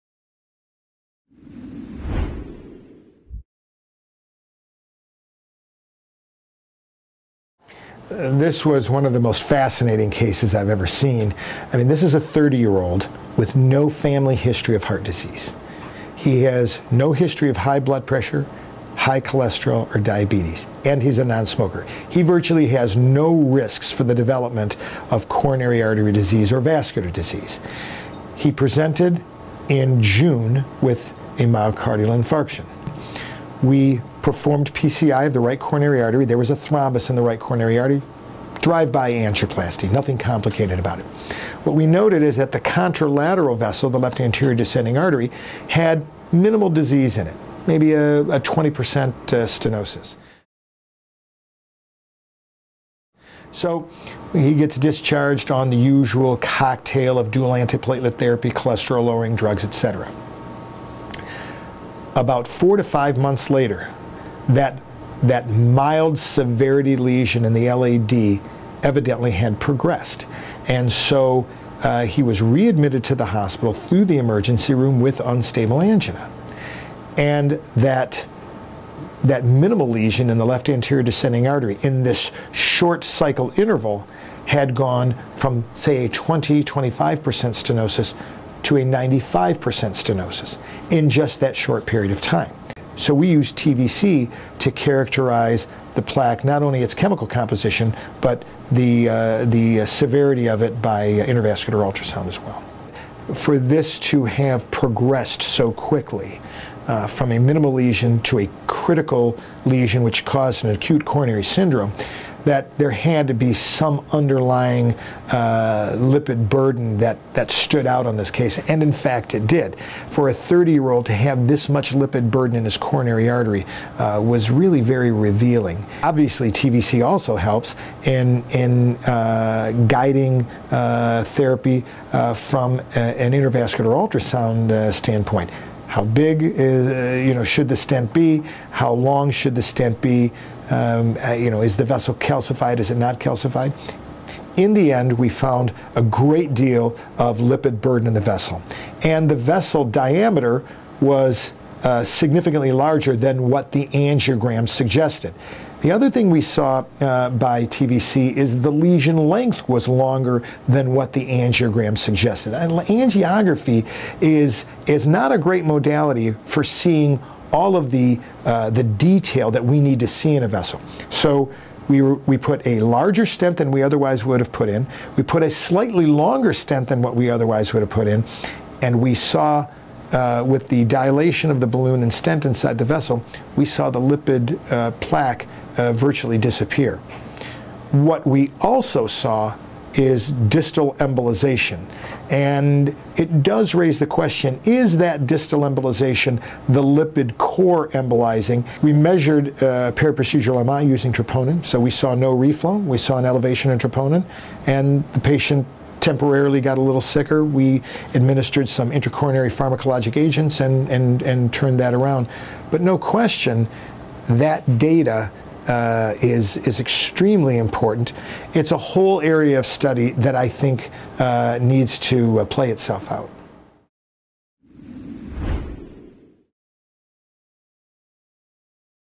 Rapid Lesion Progression with MI
Dr. David G. Rizik, Medical Director and Interventional Cardiologist at Scottsdale Heart Group describes case involving rapid lesion progression following percutaneous coronary intervention (PCI). Dr. Rizik used the TVC Imaging System to detect lipid core plaque burden and determine appropriate size and length of stent to improve outcomes.